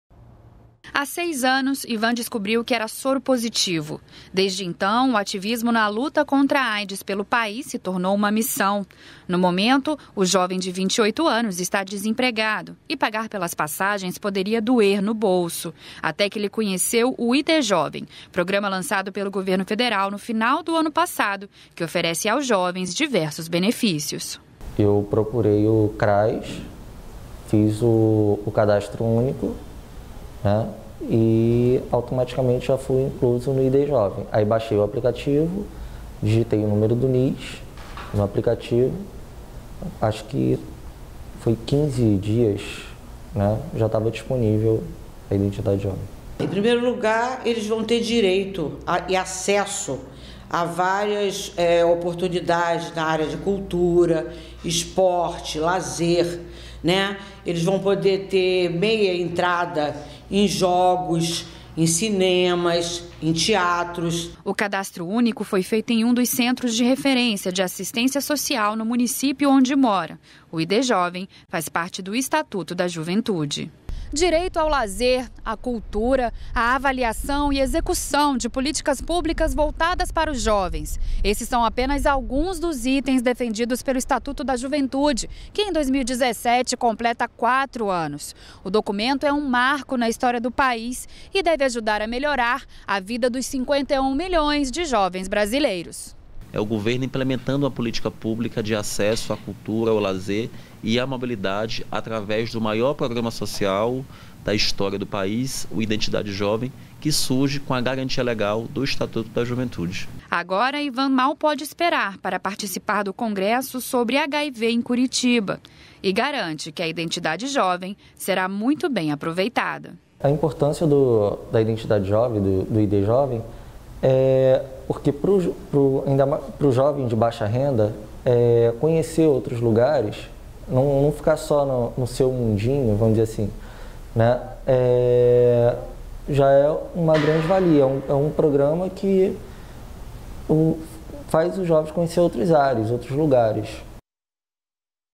Program ID Jovem garante benefícios para pessoas de baixa renda
REPÓRTER NBR - 11.08.17: Programa ID Jovem oferece benefícios para pessoas de baixa renda. O documento garante meia-entrada em eventos, gratuidade no Enem e descontos em passagens de ônibus, trem e balsas.